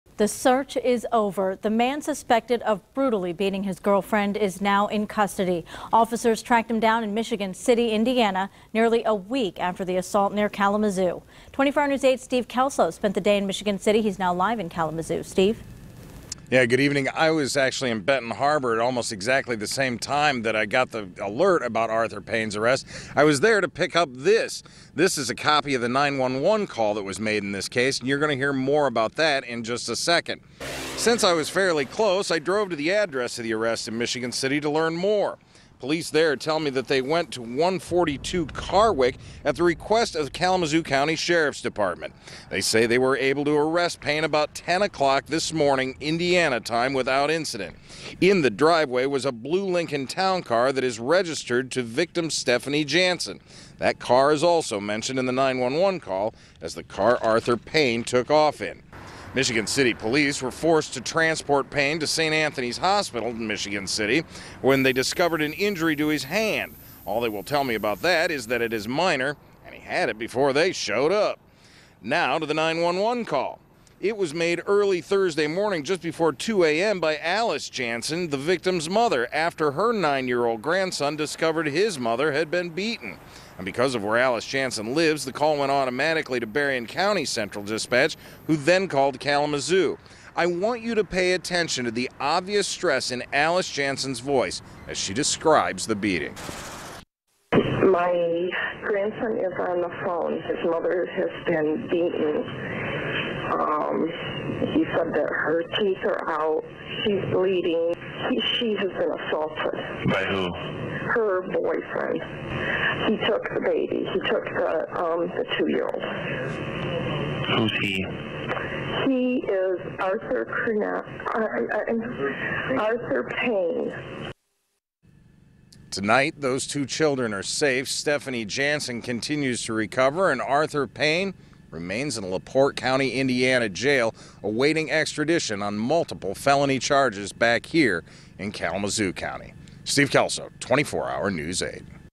Alleged girlfriend beater caught in IN
41-year-old Arthur Payne faces assault charges.